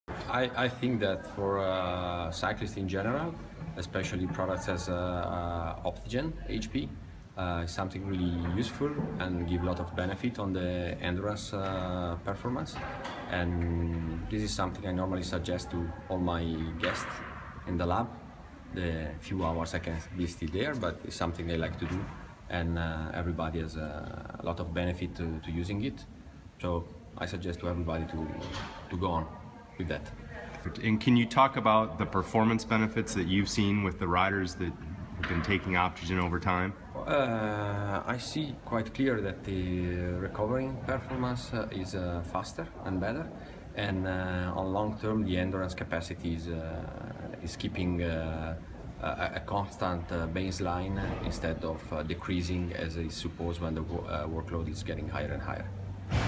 Find Out Which Endurance Supplement Luca Guercilena Recommends
Luca Guercilena , TREK General Manager & Former Mapei Sports Center Athletic Trainer, discusses why he recommends OptygenHP for every endurance athlete.